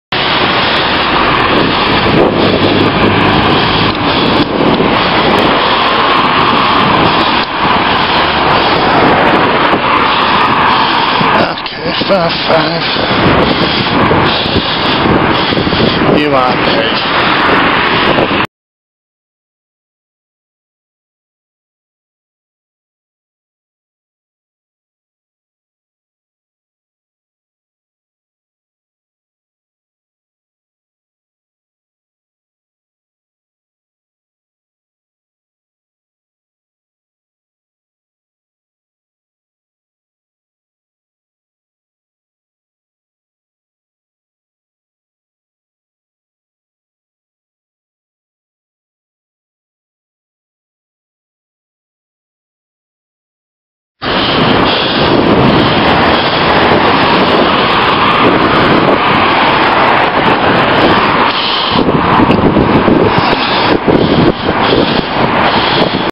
RK55URP - double close pass
The first was through a pinch point, which led me to think I was partly to blame for not controlling the lane, but the second one was as I was indicating right to avoid the dire road surface, still went for it and came too close. Incredibly incompetent.